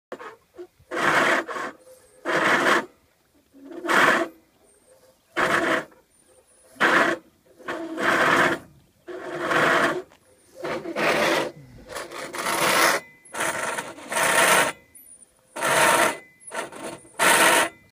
Swab hauls anchor
"Barnacle" gets promoted to "Swab" with this exercise. (I greased the bow roller after this, by the way ;).